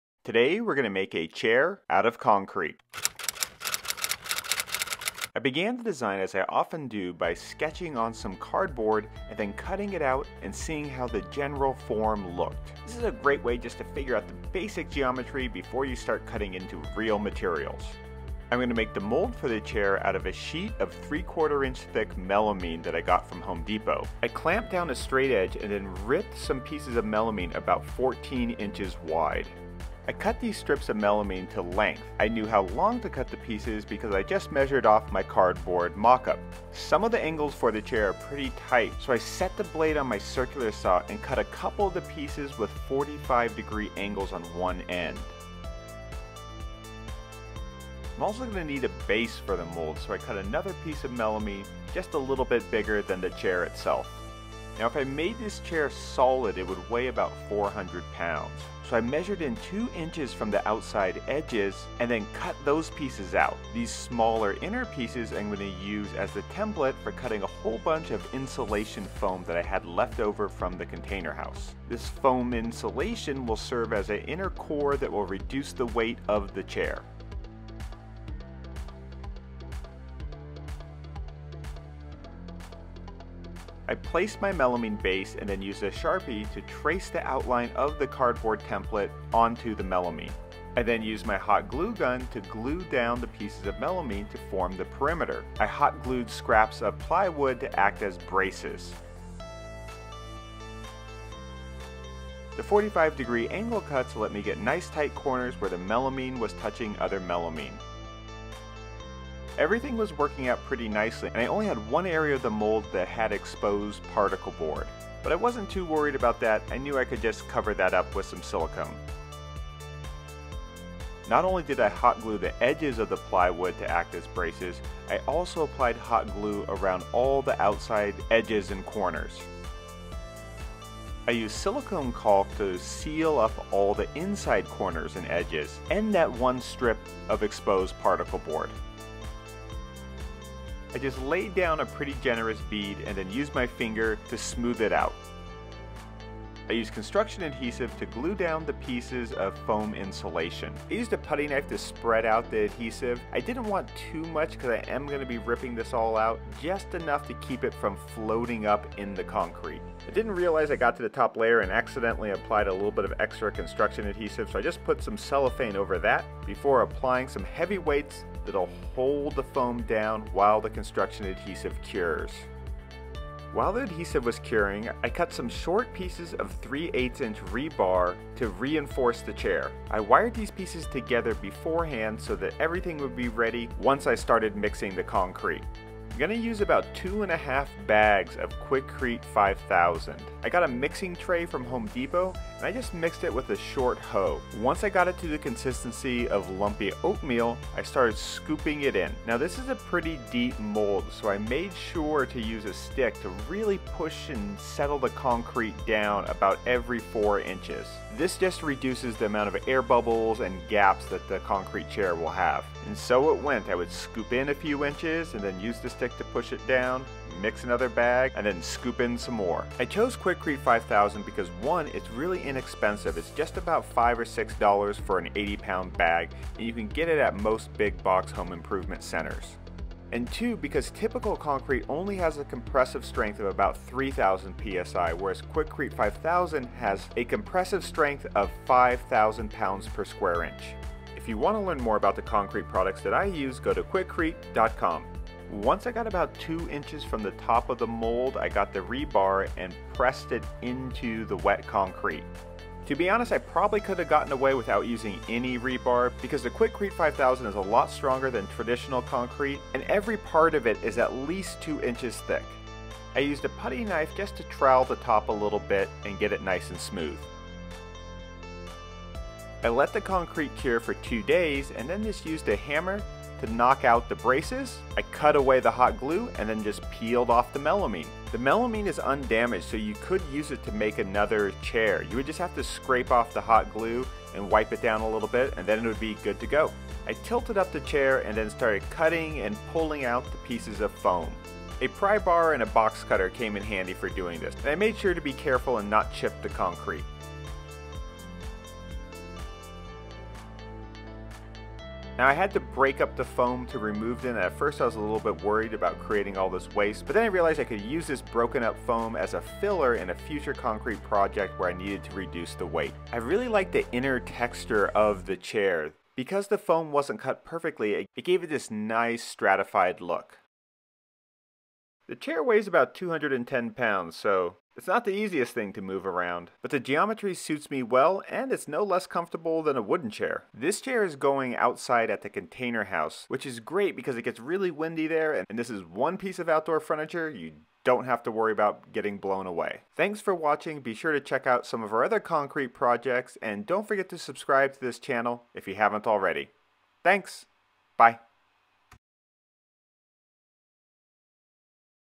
DIY Concrete Chair
In this episode I will show how to make a chair out of concrete.  I made the mold for the concrete out of 3/4" melamine that was hot glued together and then sealed with silicone caulk.  I mixed 2.5 bags of quikrete 5000 and placed it in the mold for the chair.   I used 3/8" rebar to reinforce the concrete chair.

check out Quikrete.com to learn more about the concrete products that I use.